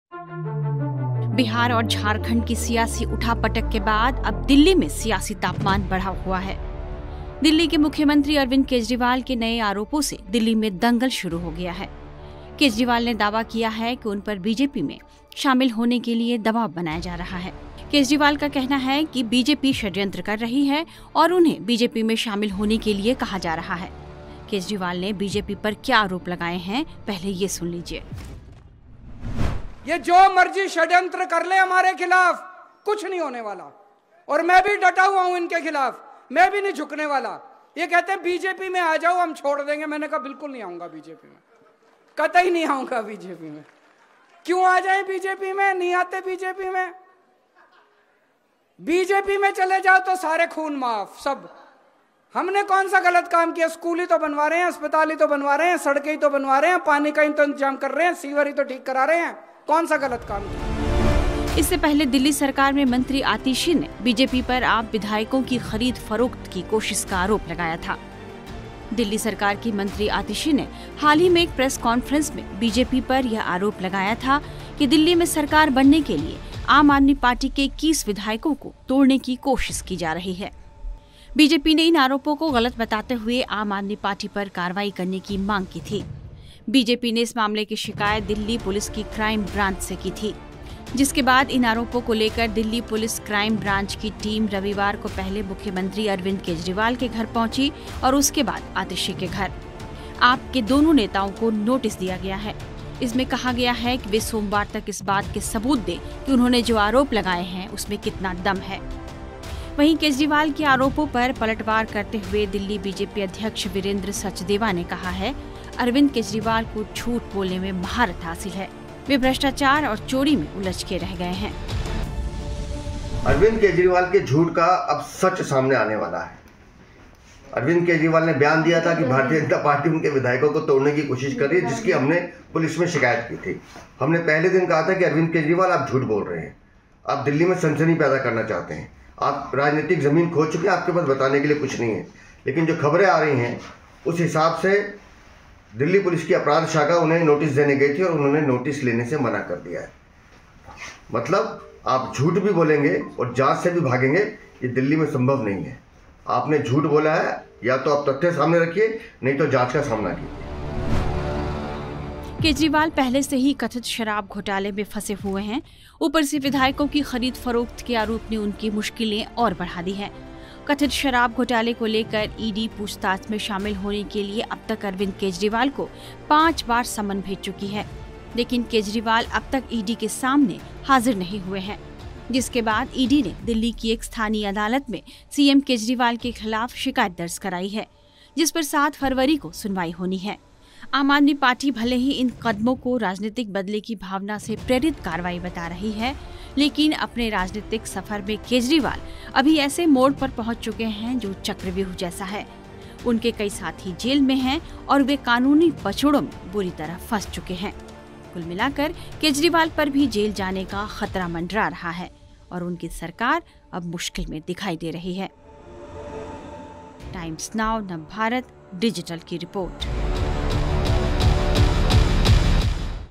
Delhi के CM Arvind Krjriwal कैसे फंसते जा रहे हैं चक्रव्यूह में ? | Hindi News
Delhi CM Arvind Krjriwal कैसे फंसते जा रहे हैं चक्रव्यूह में ? Bihar  और Jharkgand  की सियासी उठापटक के बाद अब दिल्ली में सियासी तापमान बढ़ा हुआ है.दिल्ली के मुख्यमंत्री अरविंद केजरीवाल के नए आरोपों से दिल्ली में दंगल शुरू हो गया है. केजरीवाल ने दावा किया कि उन पर ‘बीजेपी में आने के लिए दबाव’ बनाया जा रहा है. इससे पहले दिल्ली सरकार में मंत्री Atishi Marlena  ने  BJP पर आप विधायकों की खरीद-फरोख्त की कोशिश का आरोप लगाया था.बीजेपी ने इन आरोपों को गलत बताते हुए Aam Aadmi Party  पर कार्रवाई करने की मांग की थी. बीजेपी  ने इस मामले की शिकायत दिल्ली पुलिस की क्राइम ब्रांच से की थी